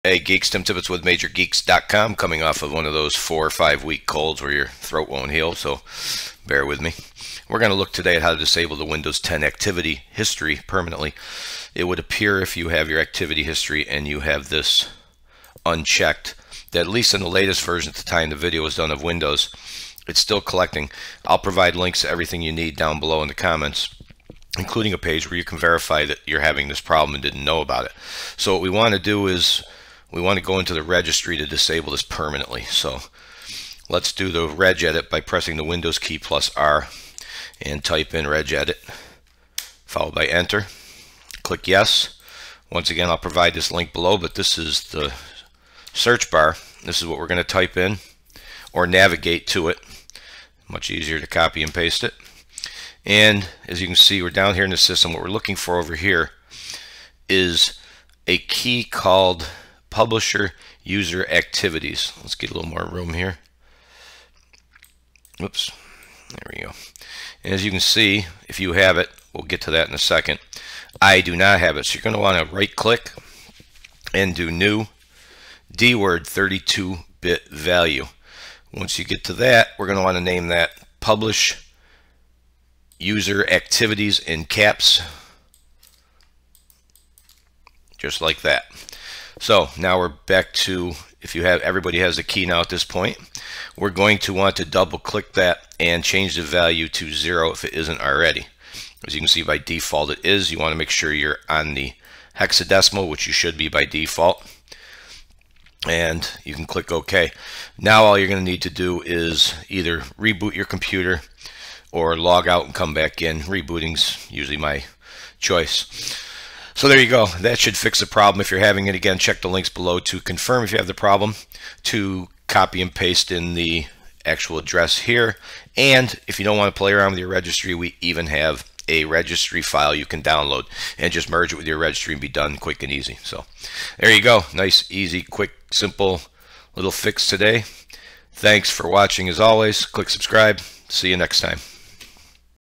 How to Disable Windows 10 Activity History Permanently
With the latest edition of Windows 10 (Redstone 5), it seems that even if you disable Activity History from PC Settings - Privacy - Activity History, Microsoft still collects activity history. Let's take care of that problem permanently.

Registry key: Computer\HKEY_LOCAL_MACHINE\SOFTWARE\Policies\Microsoft\Windows\System